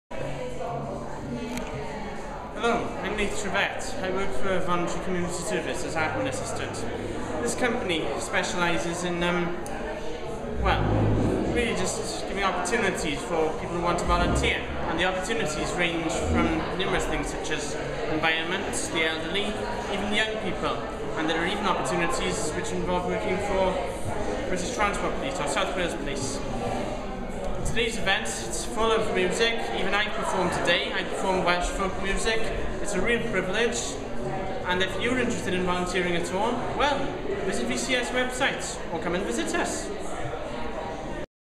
Nath Trevett
Our very own Admin Assistant Nath Trevett performed at the Mini Music Fest on 5th June for Volunteers Week. After his set, here's Nath talking about volunteering